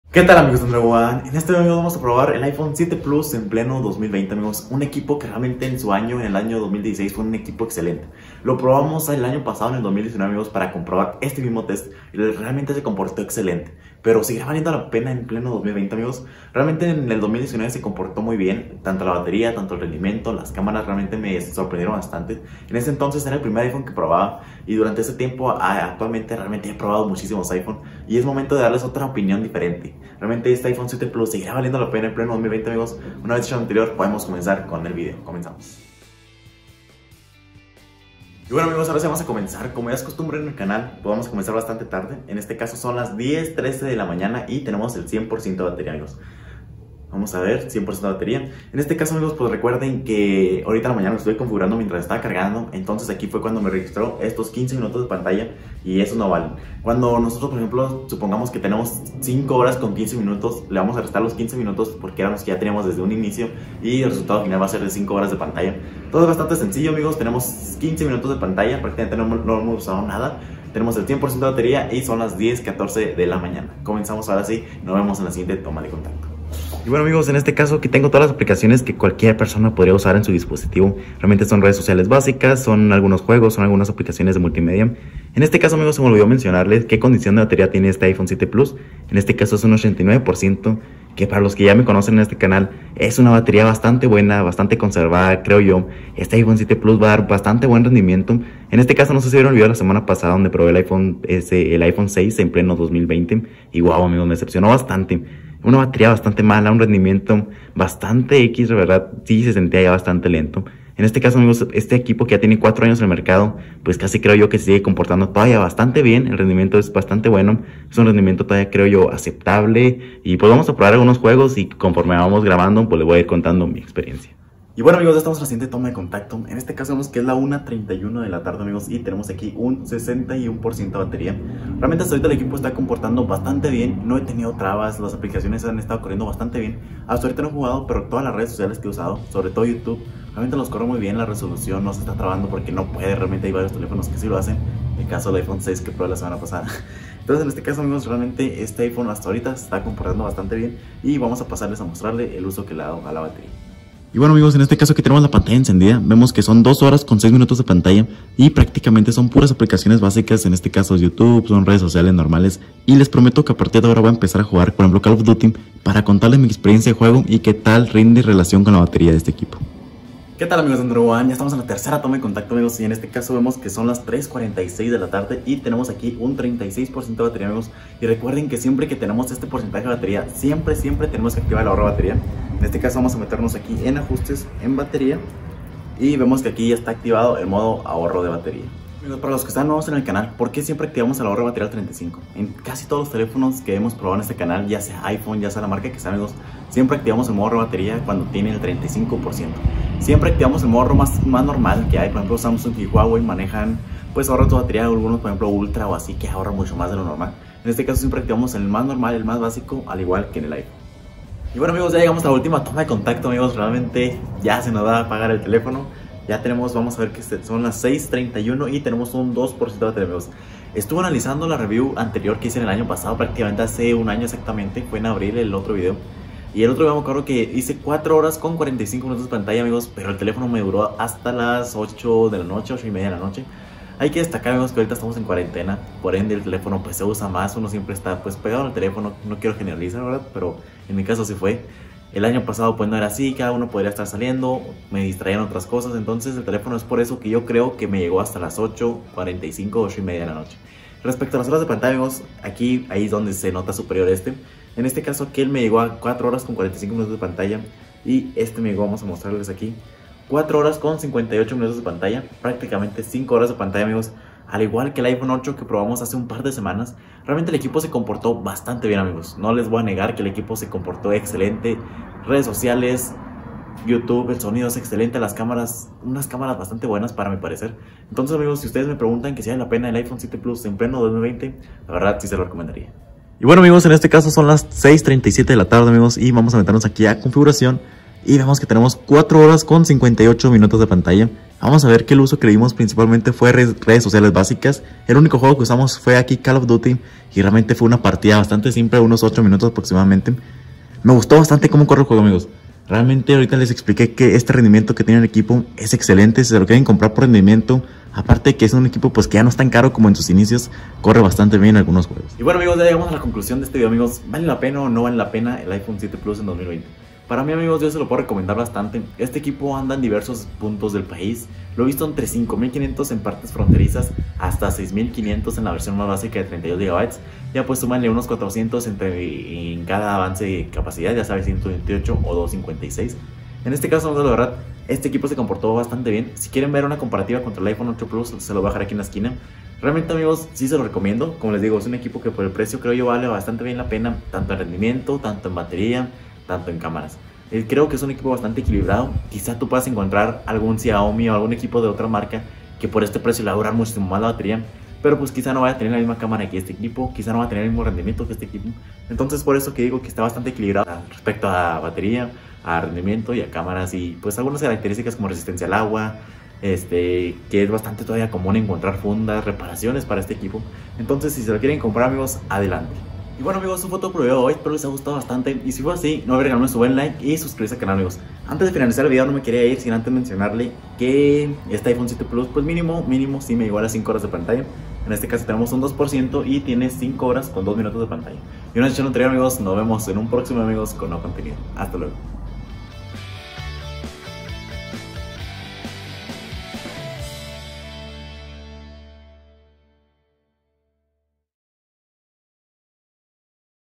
IPHONE 7 PLUS EN 2021 | TEST DE BATERÍA | AndroOne
Que tal amigos, es esta ocasión tenemos el video del Test de bateria del iPhone 7 Plus, un equipo bastante cotizado al dia de hoy por su precio y características que ofrece, un equipo de hace ya 4 años, valdrá la pena?